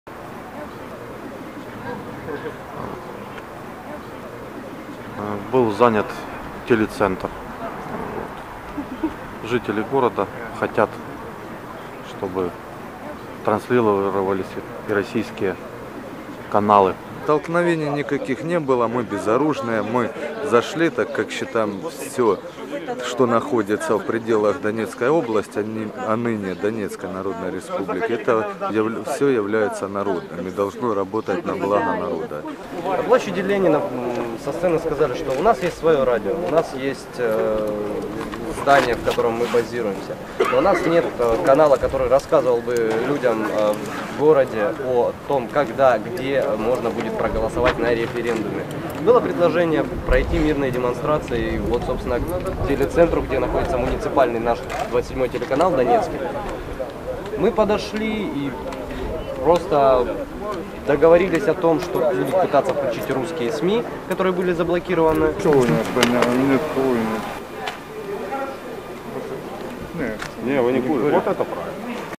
Pro-Russia Separatists Seize Donetsk TV Center
Credit: Radio Free Europe/Radio Liberty